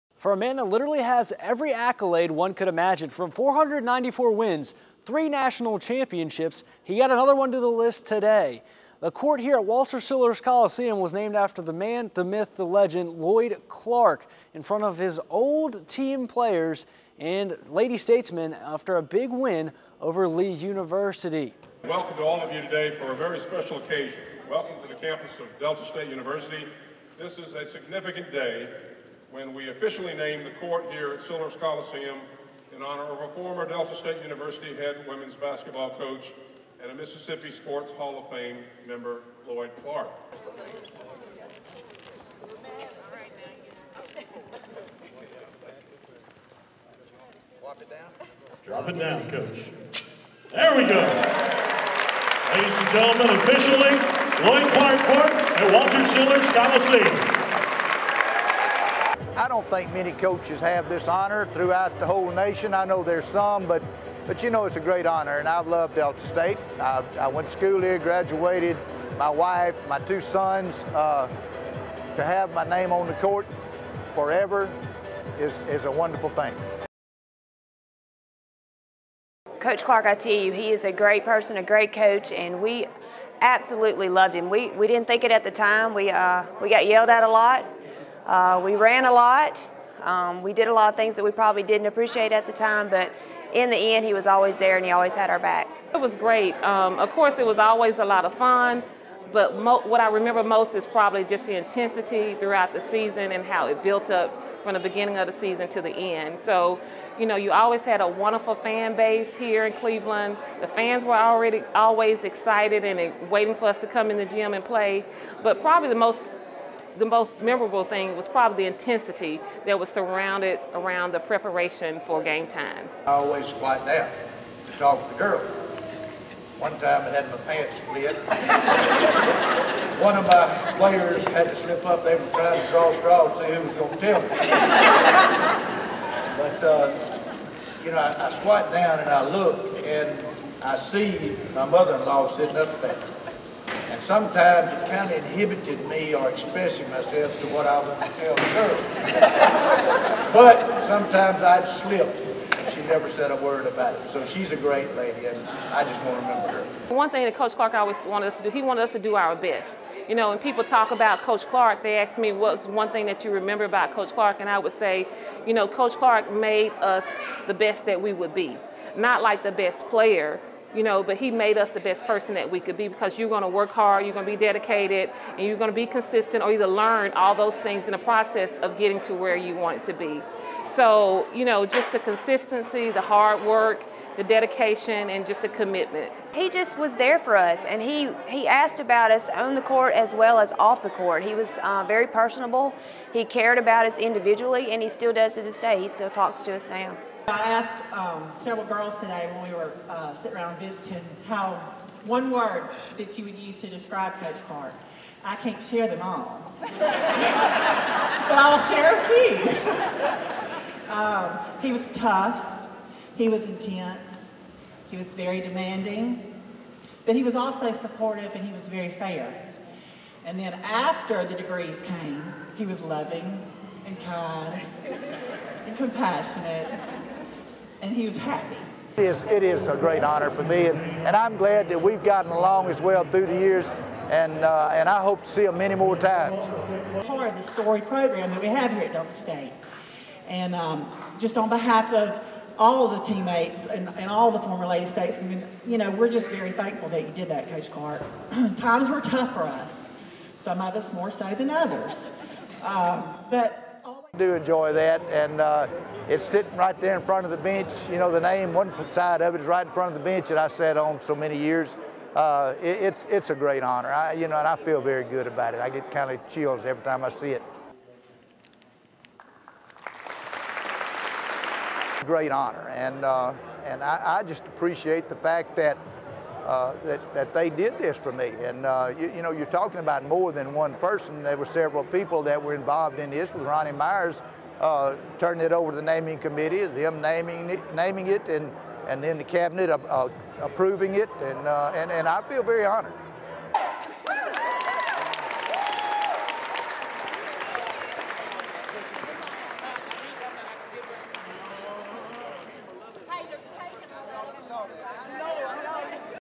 Lloyd Clark Court Dedication
Delta State University officially cut the ribbon on Lloyd Clark Court Saturday, Jan. 10 at Walter Sillers Coliseum, as the University honored the legendary women's basketball coach by naming the court in his honor. During his 19 seasons as head coach, Clark directed the Lady Statesmen to 10 Gulf South Conference titles, three NCAA Division II national championships and a 494-98 record.